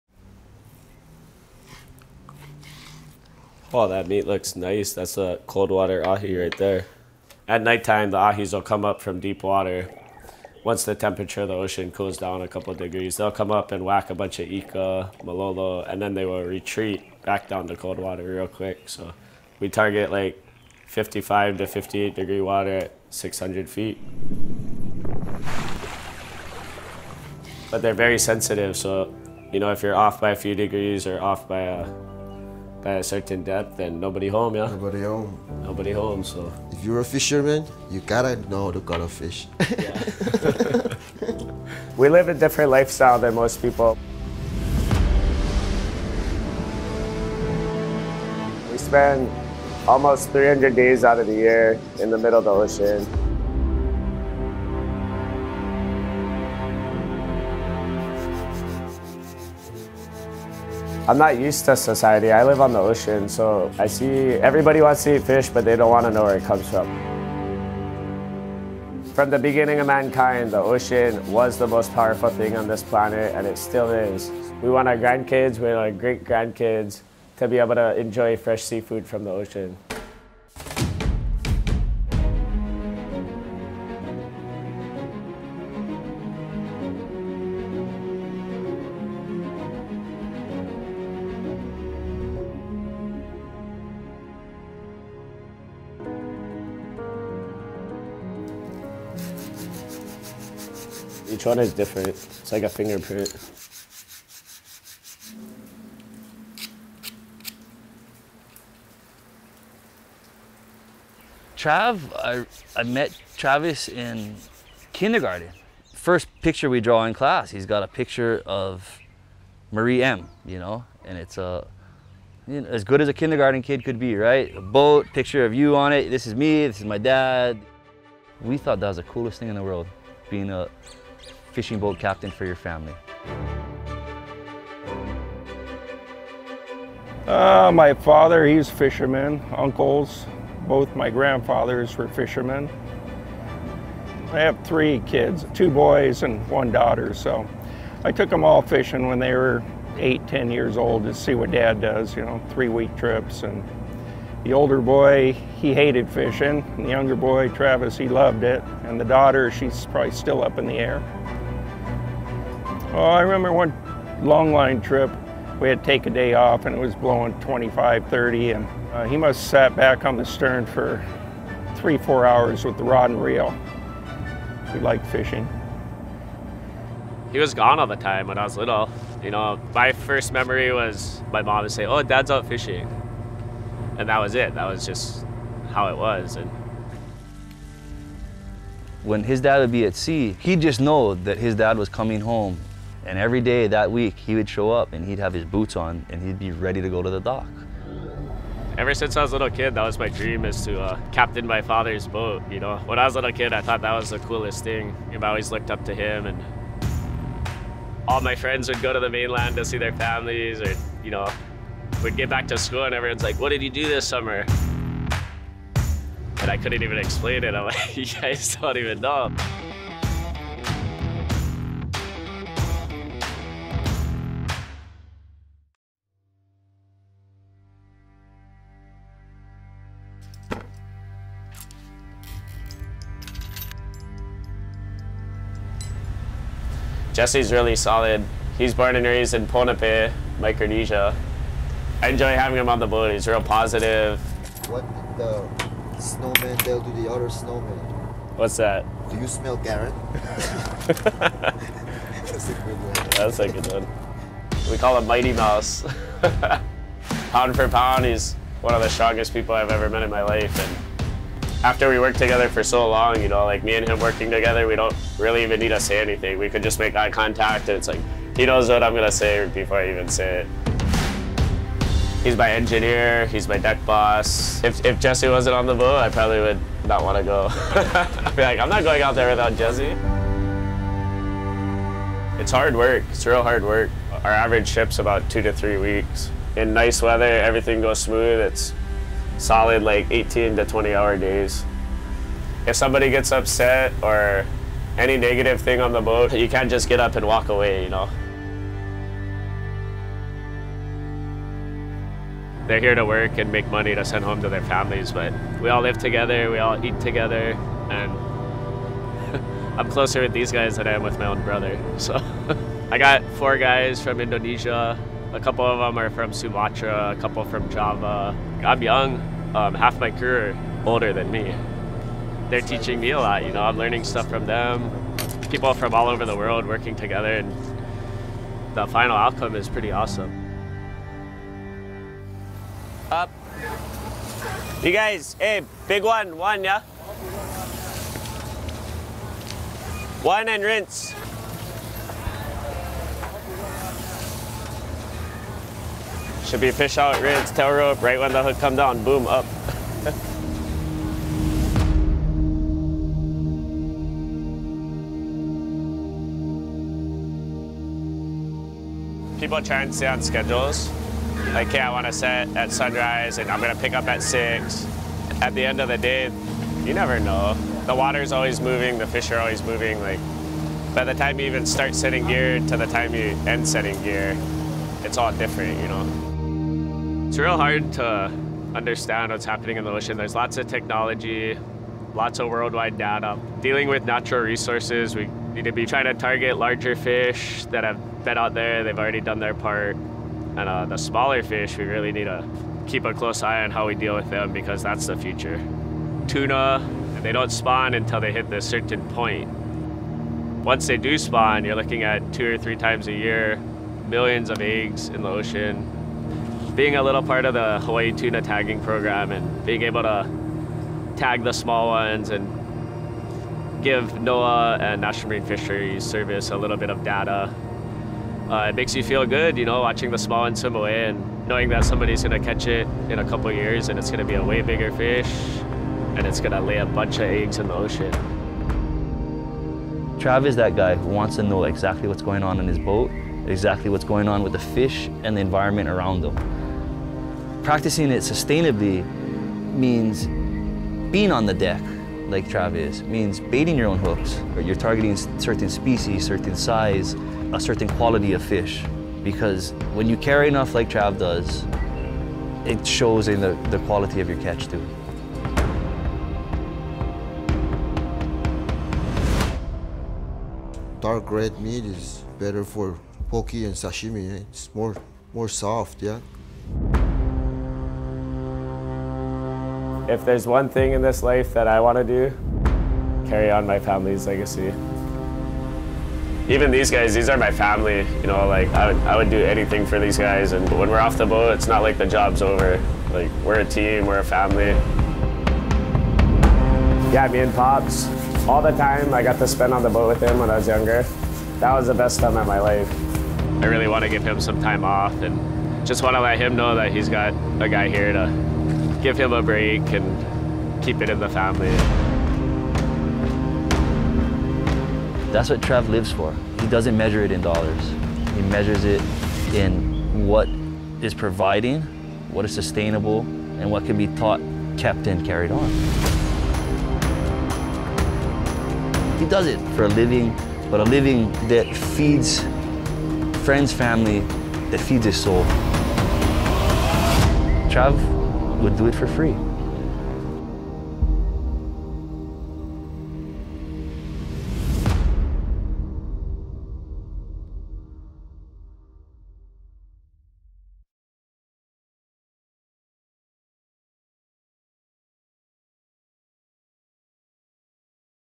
Hooked - Season 1 | Travis Myking | Hawaii Longline Association
Our latest installment in the Hooked series is a multi-generational tale that explores what it takes to become a longline captain in Hawaii.

"We live a different lifestyle than most people. We spend almost 300 days out of the year in the middle of the ocean. . .everybody wants to eat fish but they don't know where it comes from. Since the beginning of mankind the ocean has been the most powerful thing on this planet and it still is. We want our grandkids and our great grandkids to be able to enjoy fresh seafood from the ocean."